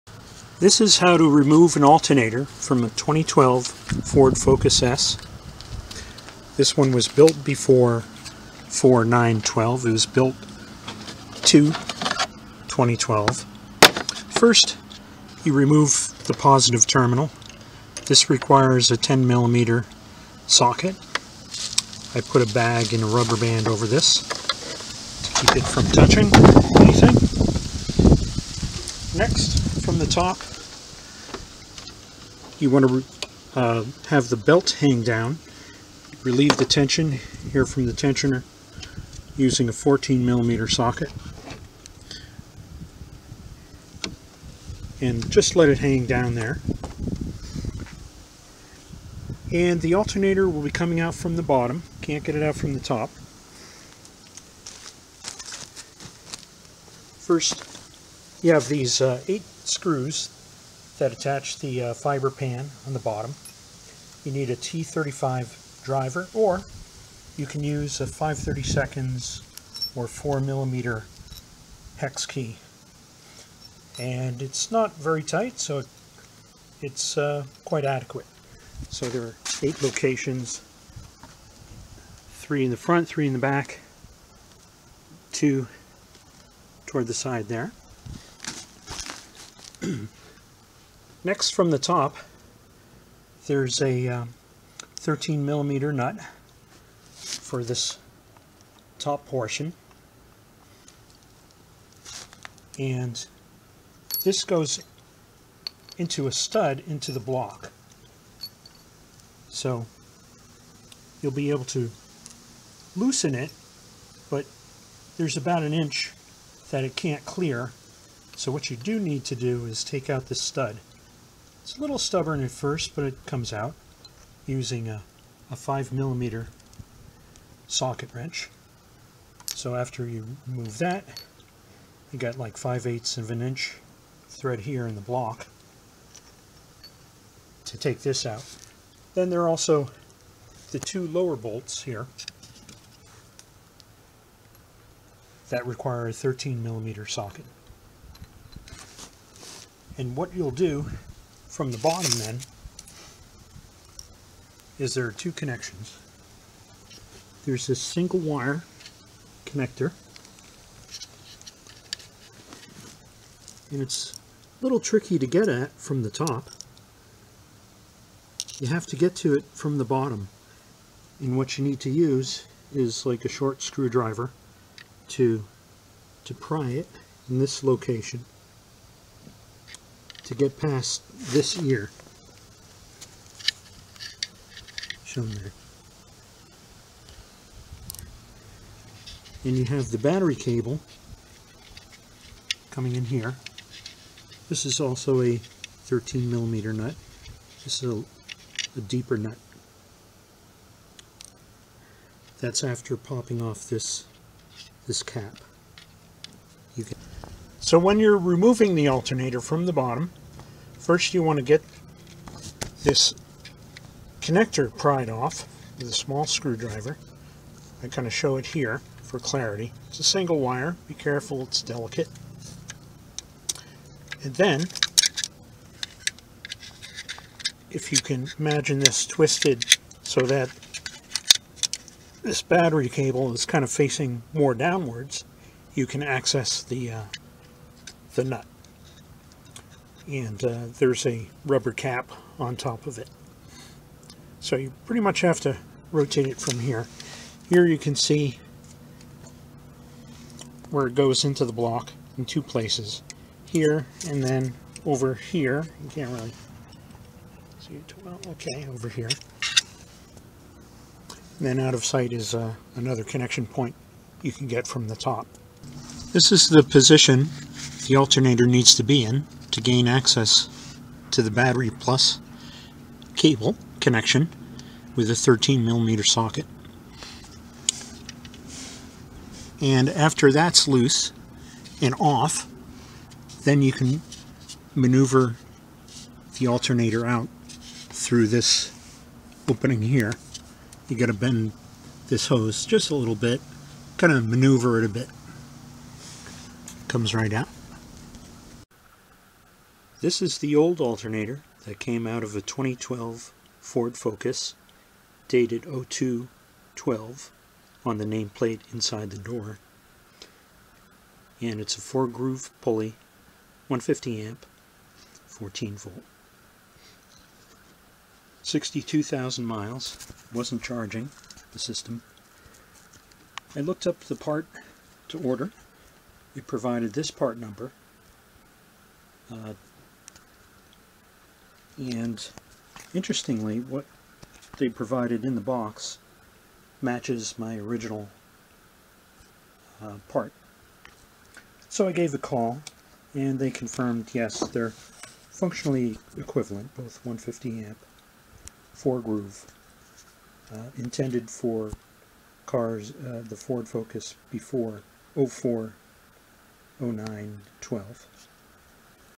2012 Ford Focus Alternator Removal
Tools and Tips for removal of the alternator on a 2012 and later Ford Focus S 2.0L DI.  Sticker on passenger side of engine cam cover indicates manufacture date of 14/02/12 (Feb 14, 2012).

Charging had been irregular since I got the car used at 22,000mi, as witnessed by headlight brightness modulating chaotically a few times per second.  Finally at 62,000mi. the battery icon came on, and system was not charging (11.8V seen continuously on battery when running).  Brushes did not look bad, armature contacts not grooved, diodes were OK.  Suspect the internal regulator/brush assembly.

On installation, be careful to make sure the bottom bolts are started squarely to prevent cross threading... the weight of the alternator tries to cock it down unless you support it.

New alternator fixed the problem (steady 15.1V on startup at 45F temp, gradually subsiding with idle RPM after a minute to 14.9V).